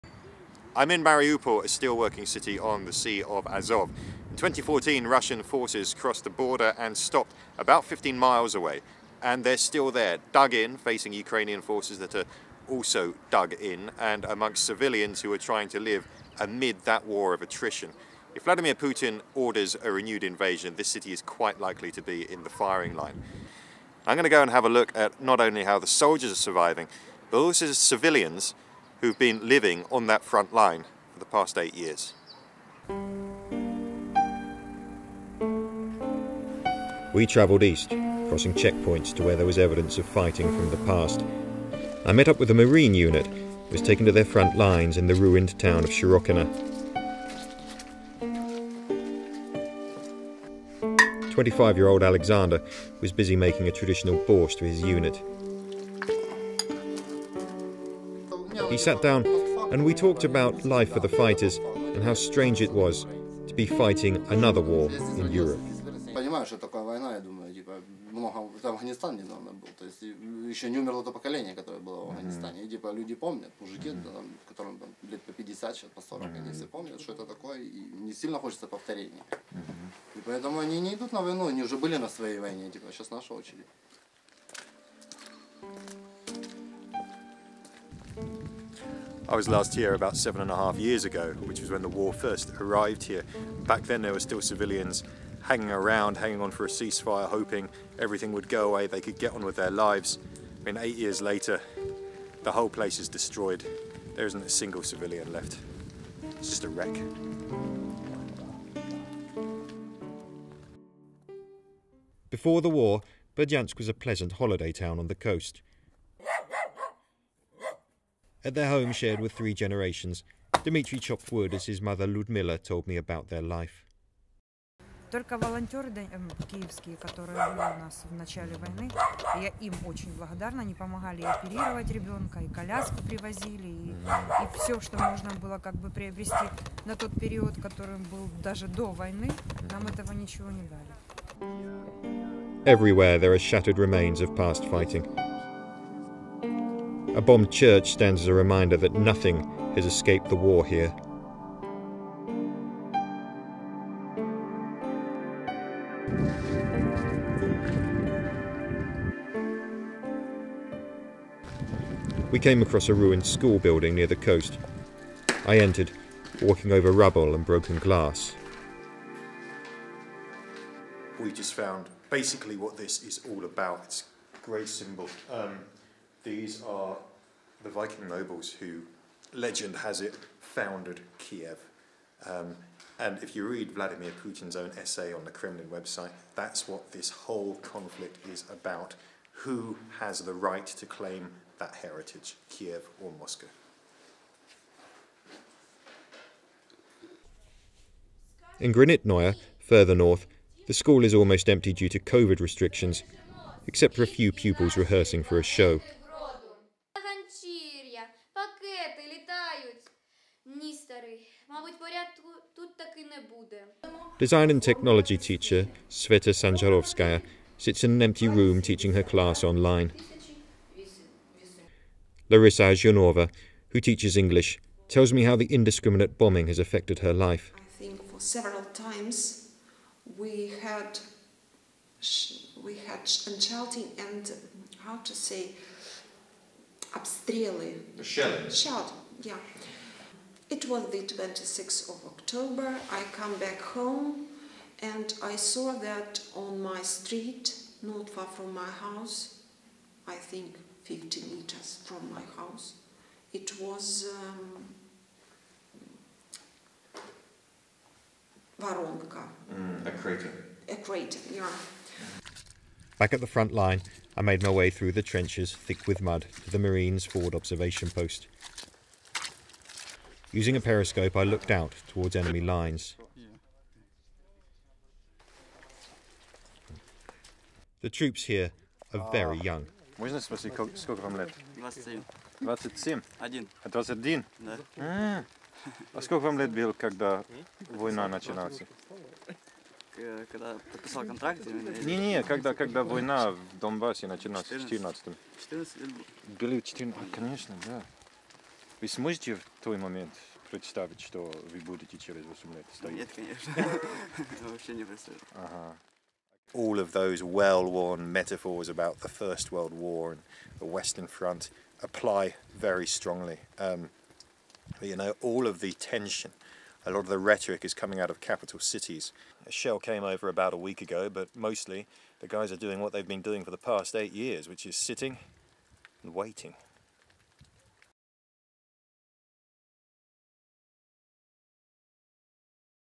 On the frontline between Russia and Ukraine: how soldiers and civilians are coping
The Telegraph's Chief Foreign Correspondent Roland Oliphant travelled with Telegraph photographer Julian Simmonds to the frontline between Ukrainian and Russian forces in the south-eastern region of Donetsk to find out how soldiers and civilians are coping as the threat of war looms.

Russia has massed upwards of 130,000 troops near its border with Ukraine and in western Belarus.

The CIA has warned Nato allies that an invasion could take place on Wednesday, following intelligence reportedly intercepted from Russian central command to troops on the ground.

The Foreign Office issued travel advice urging all British nationals to leave Ukraine.
Several other countries including Israel, Japan, Bulgaria and Germany issued similar warnings to their citizens.